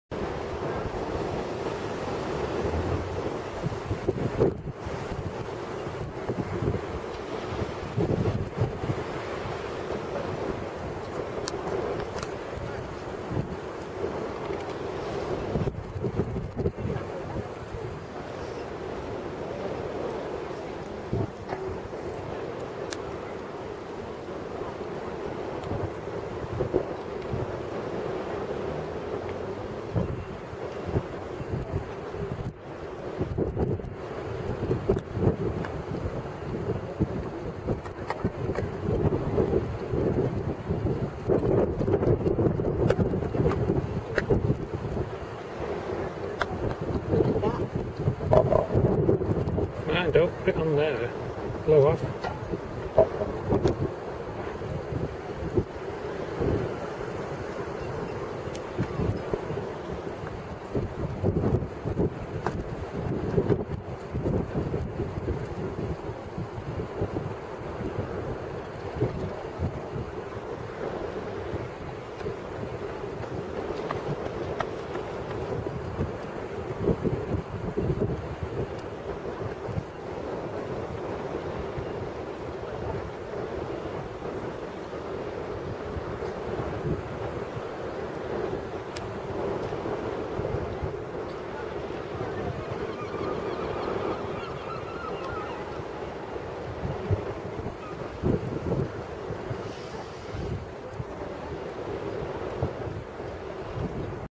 Brixham, Devon
This is Brixham Marina, another nice place to visit or stay as we did for 2 weeks, but it is very hilly you need a car to get around.